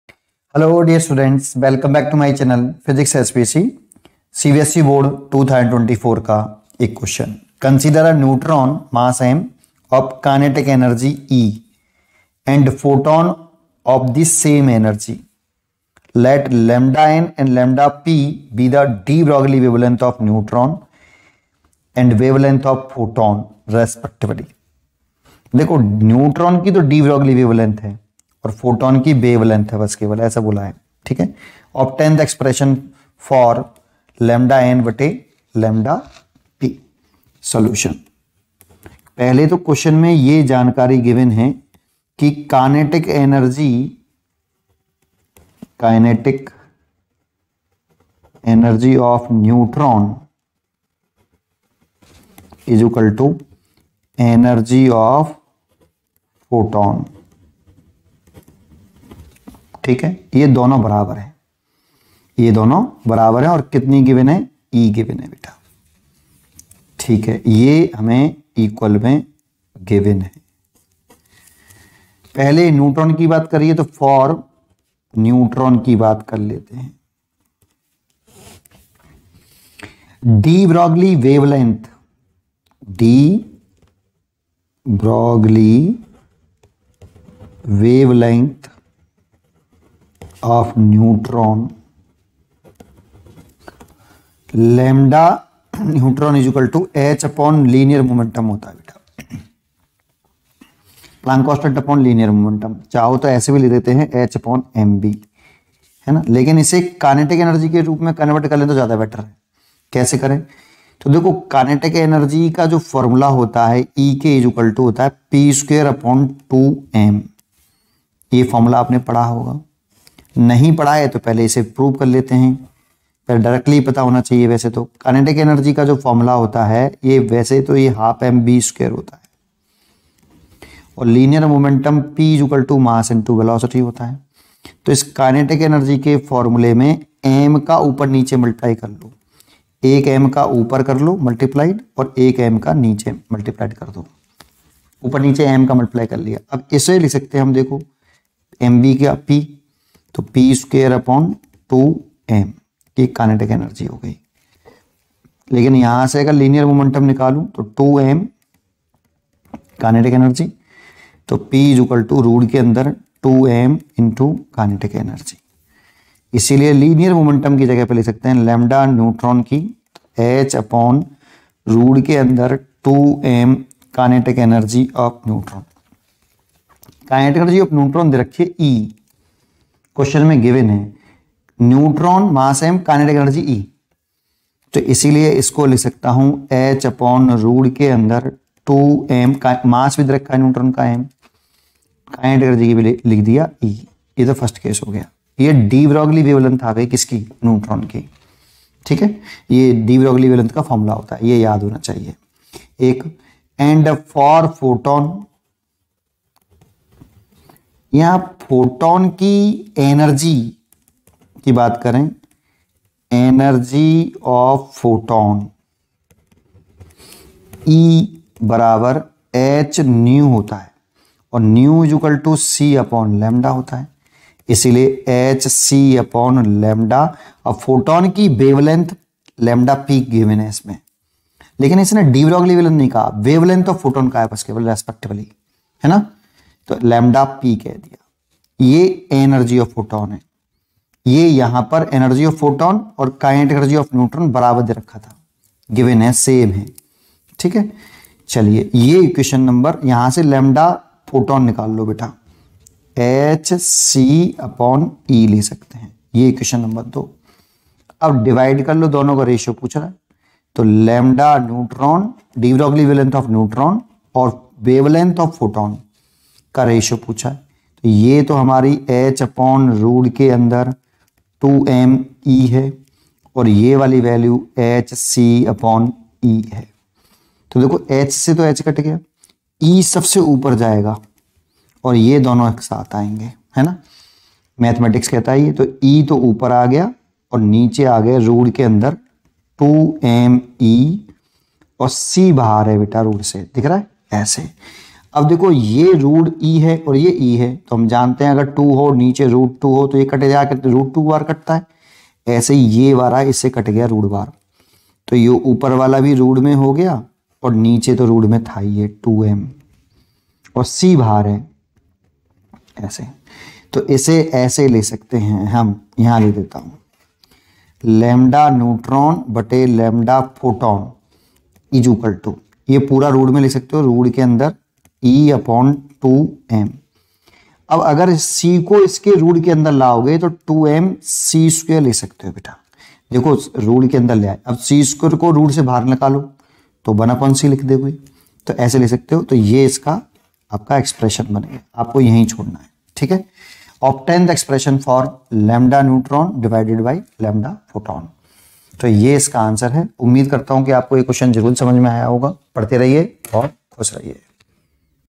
Q 20 Consider a neutron ( mass m ) of kinetic energy E and a photon of the same energy. Let ( lemma
Q 20 Consider a neutron ( mass m ) of kinetic energy E and a photon of the same energy.  Let ( lemma)n and ( lemda)p be the devBroglie wavelength of neutron and the wavelength of photon respectively.  Obtain an expression for ( lemda )n/ ( lemda ) p . [ Class 12 CBSE board 2024 physics paper Code 55/1/1 ]